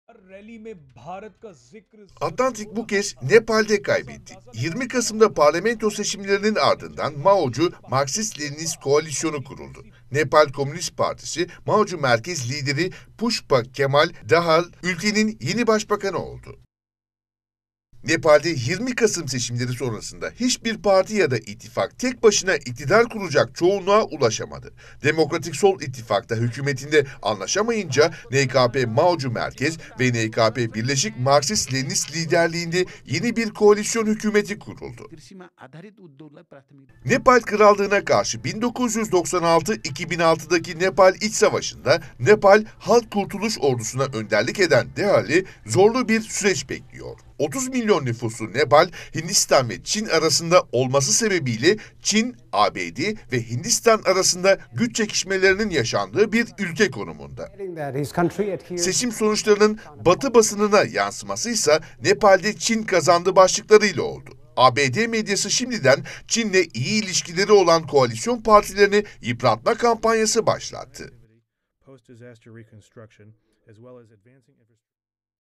ABD'ye bir darbe de Nepal'den: Yönünü Çin'e çevirdi
Nepal yüzünü Avrasya'ya döndü. Güneydoğu Asya’nın devrimci ülkelerinden Nepal’de Maocu lider Pushpa Kamal Dahal, geniş bir koalisyon hükümetinin başbakanı oldu. Nepal Devrimi’nin önderlerinden olan Dahal döneminde, önceki ABD yanlısı çizginin aksine ülkenin Çin’le ilişkilerini güçlendirmesi bekleniyor.